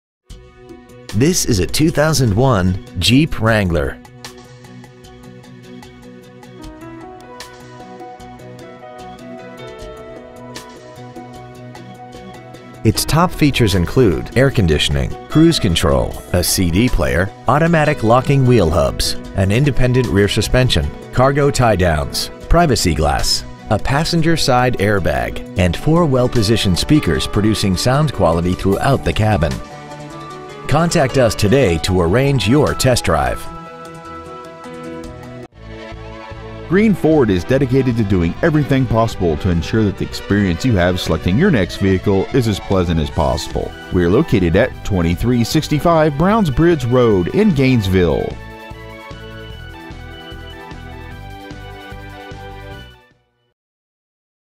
2001 JEEP WRANGLER GA
Year : 2001
 Make : JEEP
 Model : WRANGLER
 Engine : 4.0L I6
 Trans . : MANUAL
 Exterior : DARK BLUE
 Miles : 85,556
 Interior : BLACK
 Stock : P3915A

 2365 Browns Bridge Road
  , GA 30504